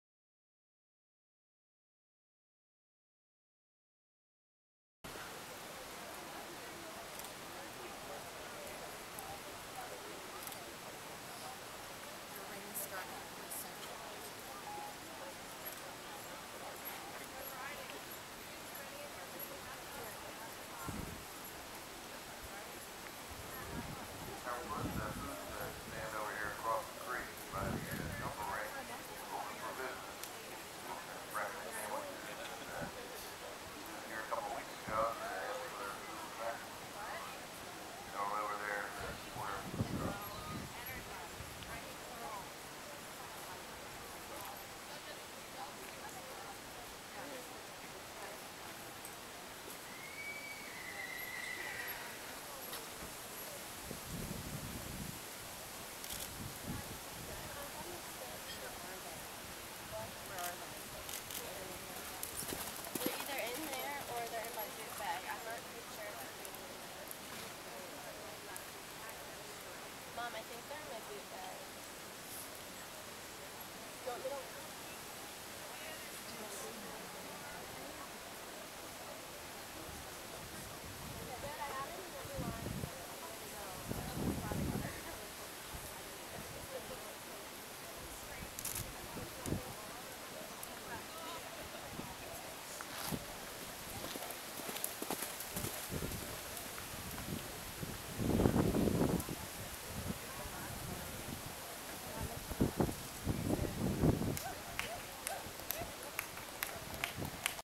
H&H Tack Shop Equitation Finals 2012
CPJHSA Horse Show

Lady was soooooooo amazinggg at this show! It was my last show on her for awhile, but I'm hoping to qualify for finals again next year! Love this pony so much(: